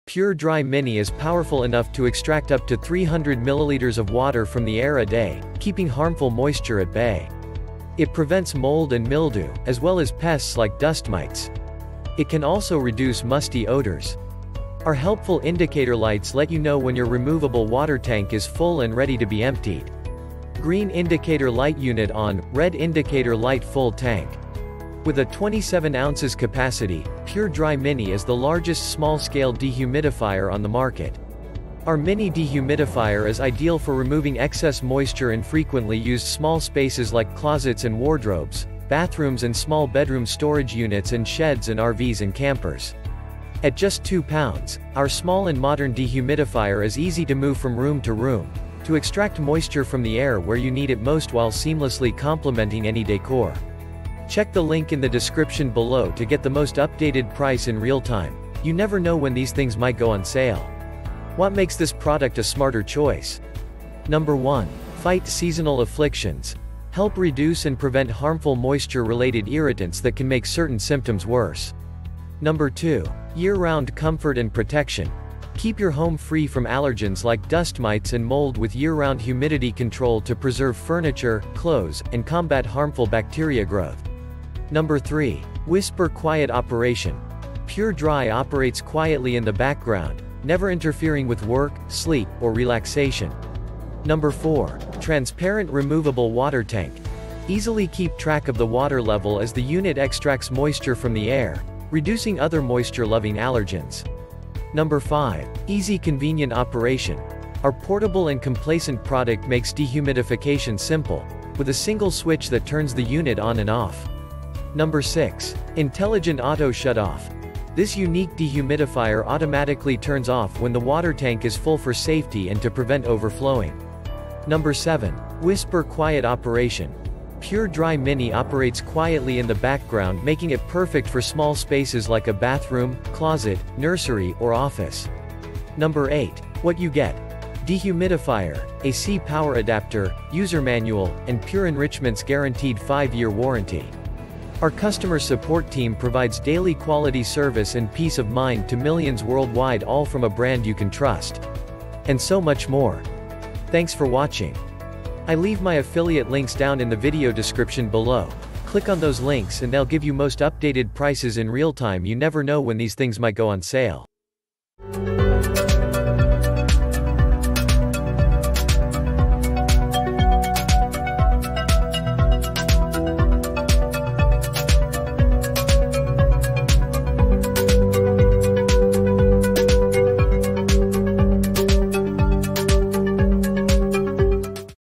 Pure Enrichment PureDry Mini Dehumidifier Review
PureDry Mini is capable of extracting up to 300 ml of water from the air every day, preventing hazardous moisture from accumulating. Mold and mildew, as well as pests like dust mites, are kept at bay. It may also help to get rid of musty smells. When your detachable water tank is full and ready to be emptied, our handy indicator lights let you know. The green indicator light indicates that the device is turned on, while the red indicator light indicates that the tank is full. PureDry Mini is the biggest small-scale dehumidifier on the market, with a 27 oz capacity. Our little dehumidifier is perfect for eliminating excess moisture from closets and wardrobes, bathrooms and small bedrooms, storage facilities and sheds, and RVs and campers. Our tiny and contemporary dehumidifier, weighing just 2 lbs, is simple to carry from room to room, extracting moisture from the air where you need it most while blending in with any design.

Number One: Fight Seasonal Afflictions: Assist in the reduction and prevention of dangerous moisture-related irritants that may aggravate specific symptoms.

Number 2: Year-Round Comfort and Protection: Use year-round humidity management to keep your house free of allergens like dust mites and mould, as well as to preserve furniture and clothing and battle hazardous bacteria development.

Whisper is number three. PureDry runs silently in the background, never interfering with your work, sleep, or leisure.

Number 4: REMOVABLE TRANSPARENT WATER TANK: As the device removes moisture from the air, decreasing other moisture-loving allergies, you can easily keep track of the water level.

Number 5: EASY CONVENIENT OPERATION: With a single switch that switches the machine on and off, our portable and complacent equipment makes dehumidification easy.

Number 6: INTELLIGENT AUTO SHUT-OFF: For safety and to avoid overflowing, this innovative dehumidifier automatically switches off when the water tank is full.

WHISPER-QUIET OPERATION NUMBER 7: PureDry Mini is ideal for tiny locations such as a bathroom, closet, nursery, or workplace since it runs silently in the background.

Number eight. WHAT YOU GET: A dehumidifier, an AC power adapter, a user handbook, and Pure Enrichment's 5-Year Guarantee. Our customer service staff offers everyday excellent service and peace of mind to millions of people throughout the globe, all from a trusted brand.

Searches related to Pure Enrichment PureDry Mini Dehumidifier Review